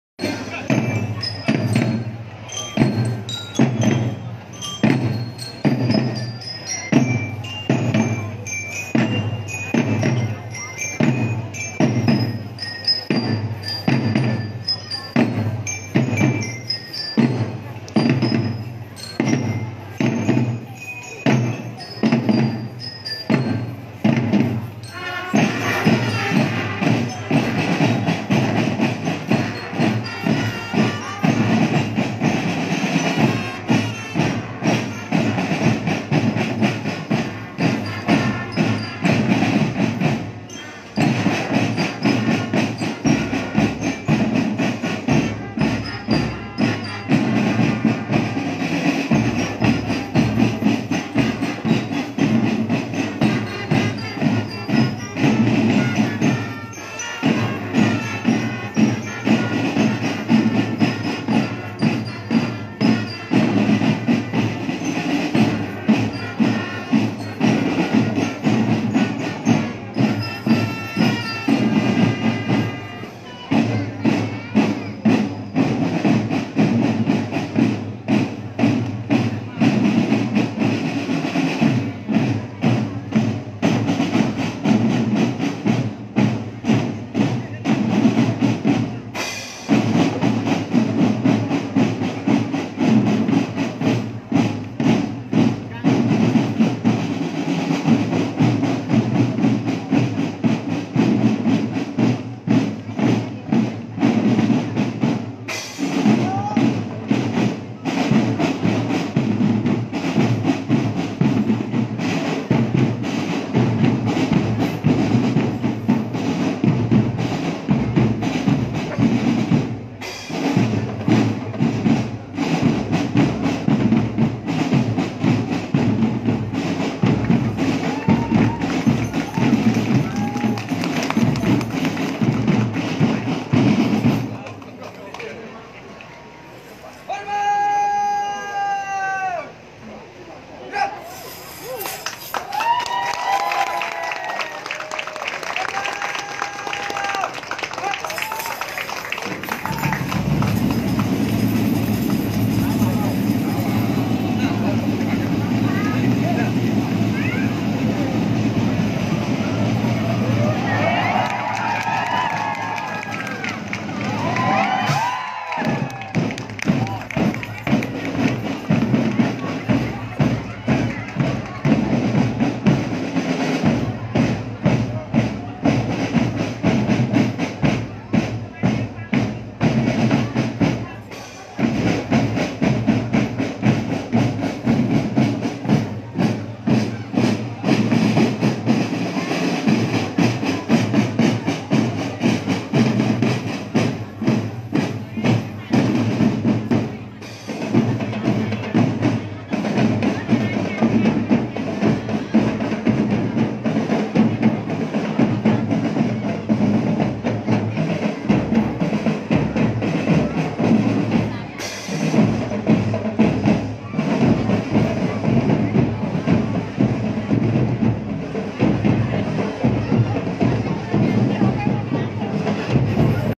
BEGINI PENAMPILAN TARUNA AKEDEMI KA MADIUN
SURYA Online | Aksi siswa siswi taruna Ademi Perkeretaapian Indonesia (API) dalam acara Jalan Sehat BUMN Hadir untuk Negeri